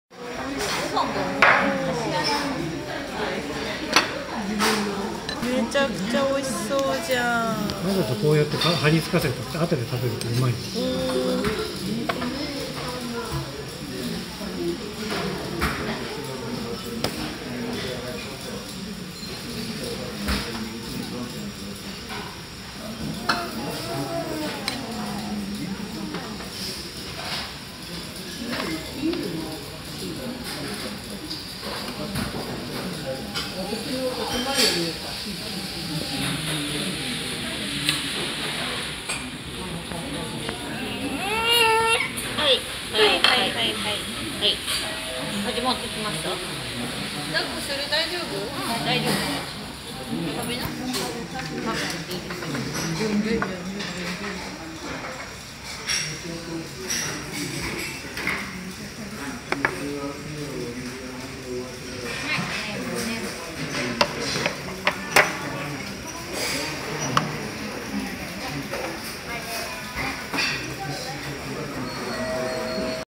和幸 入間東町店でランチ
個々に炊いた
釜炊きごはんがすごくおいしかったです！